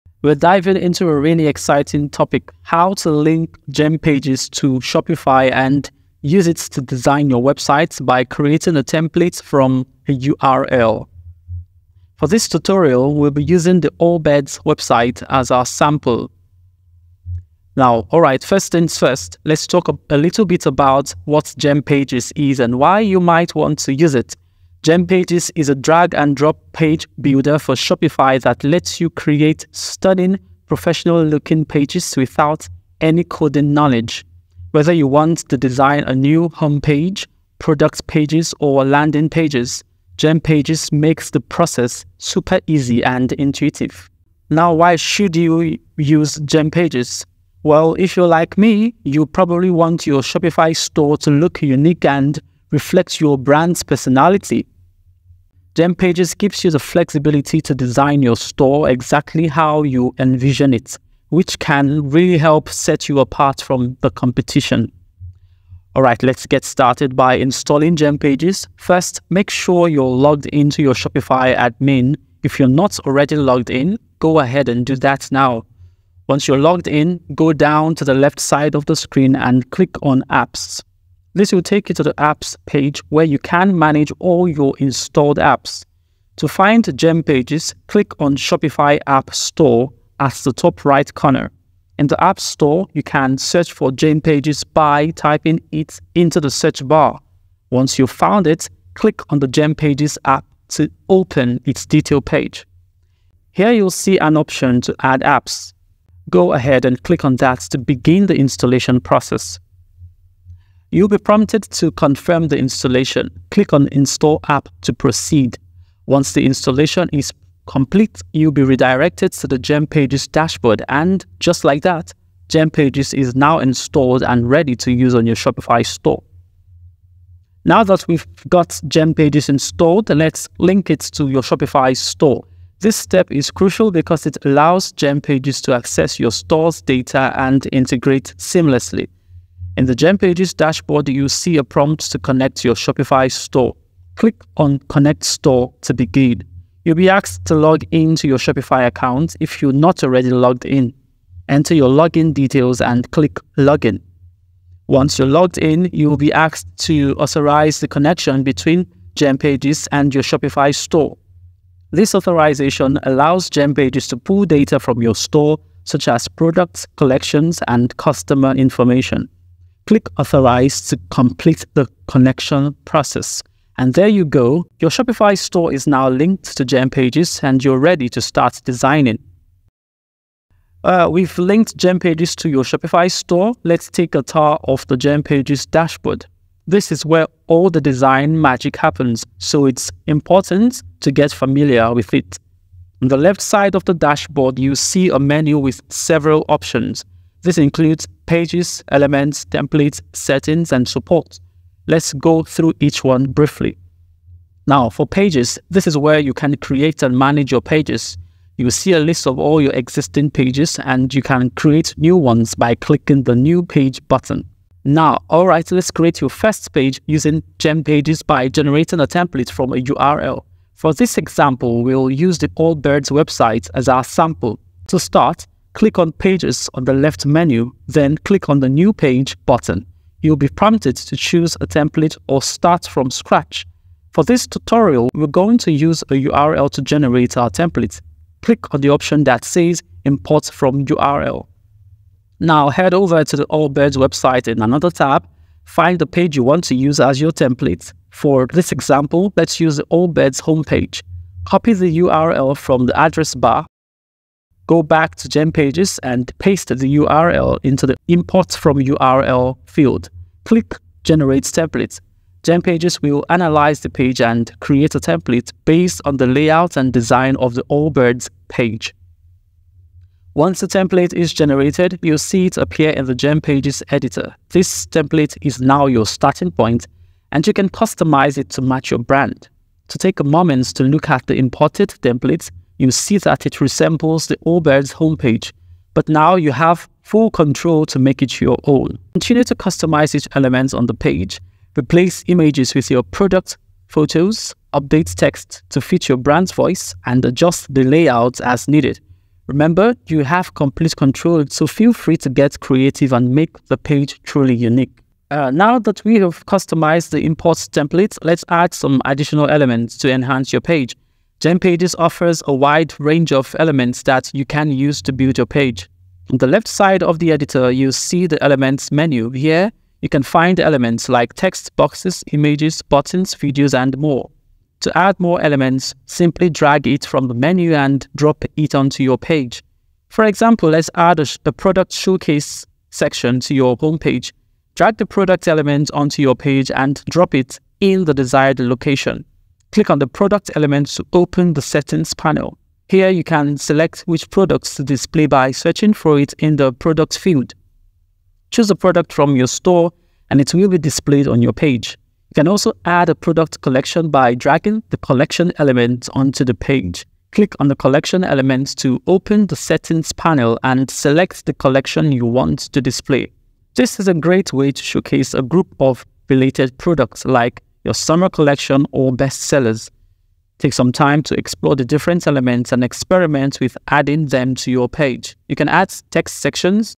Gempages Shopify Tutorial 2024 | How To Use Gempages For Shopify
Creating a Shopify website is now simpler than ever. In this video, I will guide you through the entire process of designing your Shopify store with detailed, step-by-step instructions, so you can easily follow along.